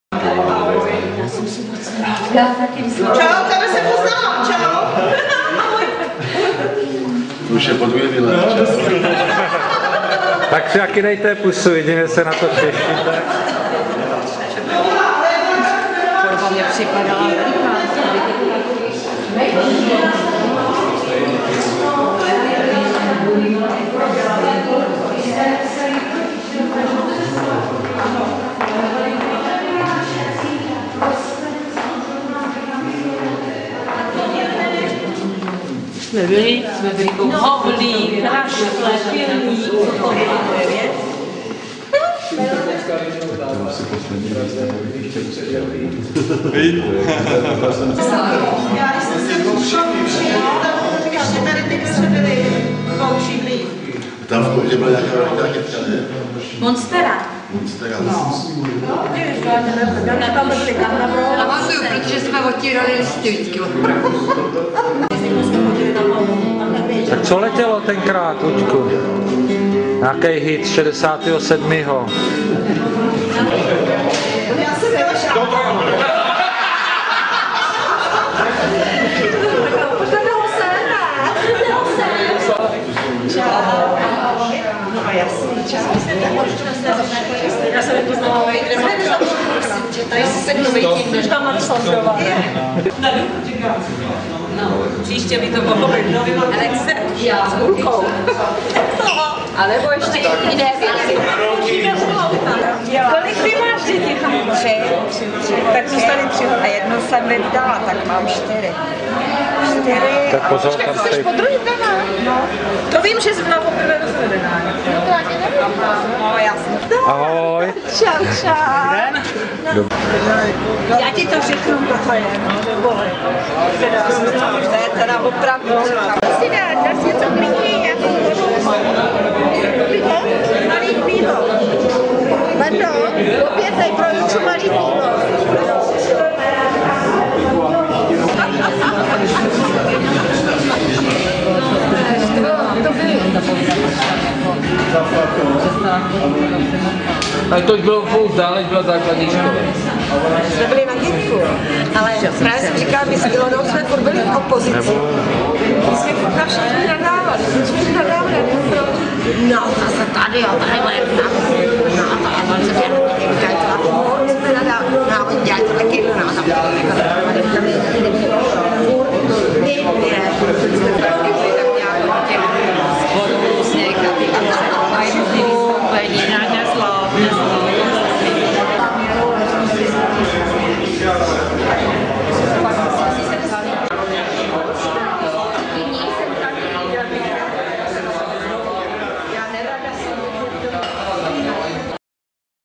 Spolužáci z komendy 1968/2008
Spolužáci z Ústí nad Orlicí ze ZDŠ Komenského  9.B, kteří absolvovali v roce 1968. Sešli jsme se v roce 2008 a navštívili jsme  naši školu a pak jsme poseděli
v hospodě na Hutvaldě.